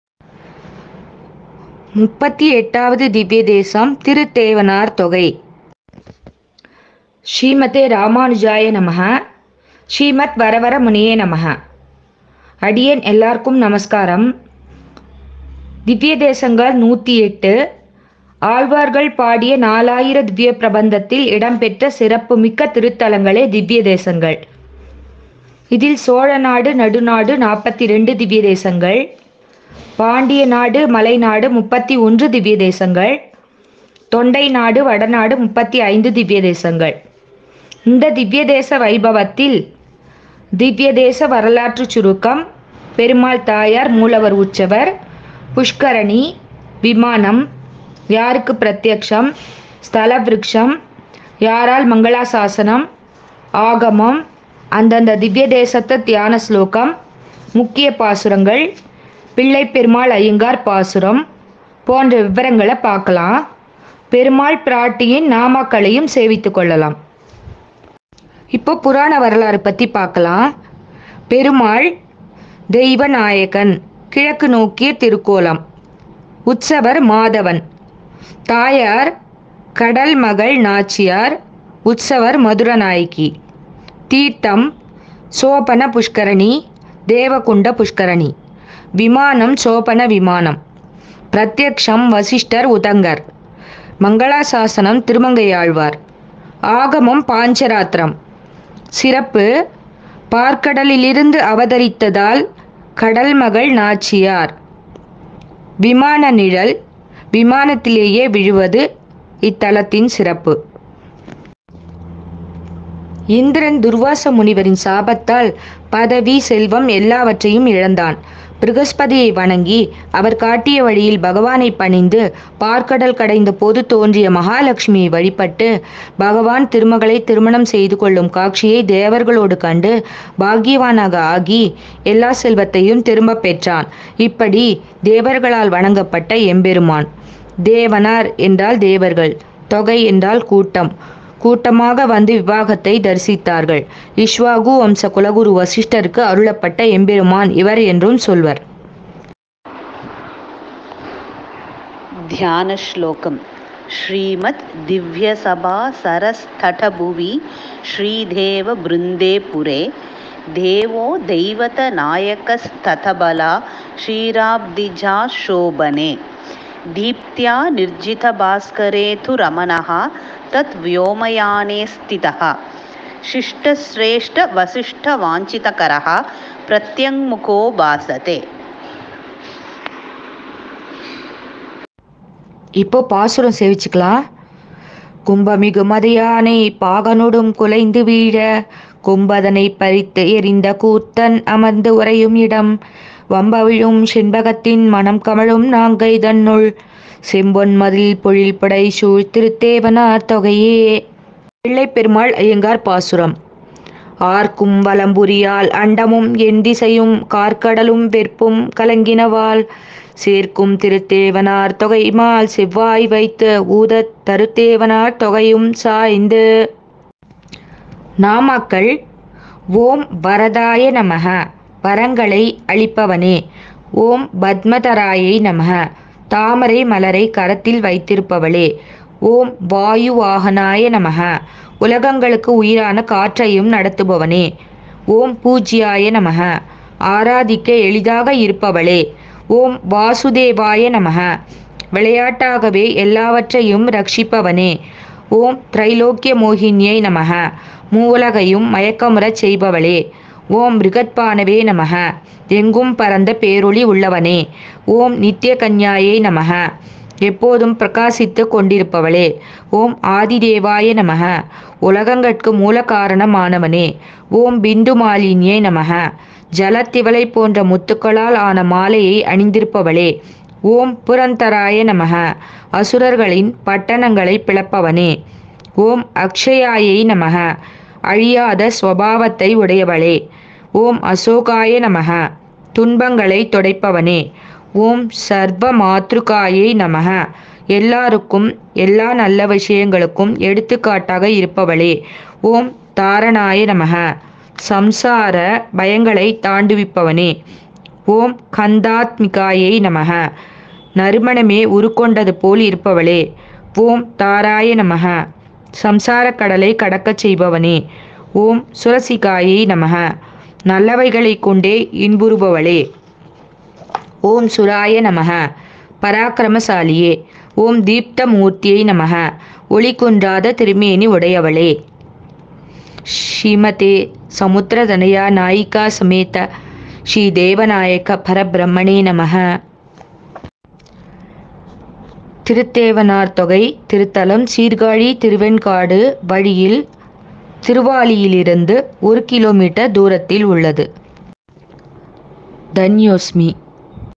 38 Devanar Thogai | திருத்தேவனார் தொகை | ஸ்ரீ தெய்வ நாயக பெருமாள் | Smt Malini Bashyam T Nagar
108 திவ்ய தேச குறிப்பு | 38 Devanar Thogai | திருத்தேவனார் தொகை (கீழச்சாலை) ஸ்ரீ தெய்வ நாயக பெருமாள் ஸ்ரீ கடல்மகள் நாச்சியார் | Smt Malini Bashyam T Nagar